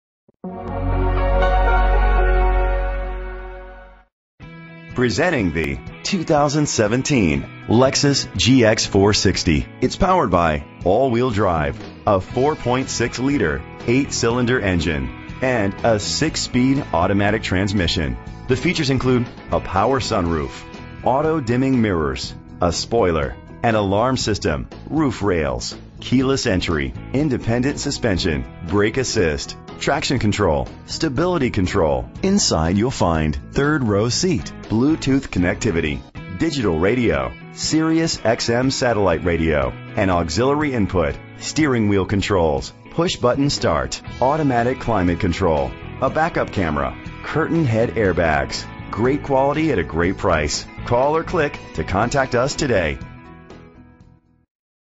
2017 Lexus GX 460 for sale in Raleigh NC
2017 Lexus GX 460 for sale in Raleigh NC - Stock # 52107
Year: 2017
Make: Lexus
Model: GX 460
Trim:
Engine: 4.6 liter 8 Cylindercylinder AWD
Transmission: 6-Speed Automatic
Color:
Mileage: 9
Address:
VIN:JTJBM7FX9H5166250